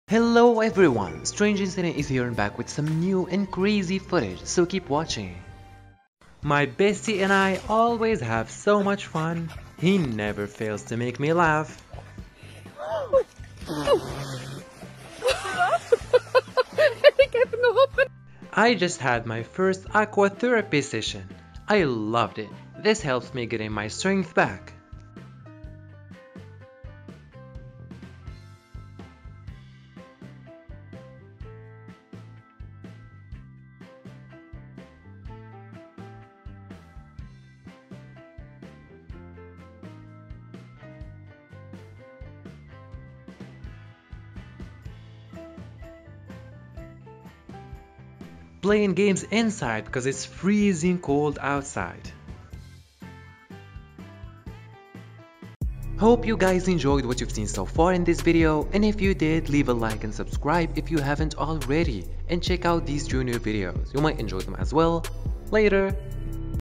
Clumsy French Bulldog Accidentally Falls Into Stream
This video contains 3 clips of a super funny French Bulldog puppy named Grizzly. In the 1st clip of the video we have Grizzly running and playing around a river in the woods but for an instant he goes up and rolls from a really high place and accidentally falls down into the river and keeps the play like nothing has happened, this funny french bulldog never fails to make me laugh. 2nd clip we have Grizzly doing his first aqua therapy session at Totalfit Dierfysio, and he looks like all he wants is the treat in front of him. 3rd clip of it is where the weather is cold outside and he wants to play so he can do nothing but play inside.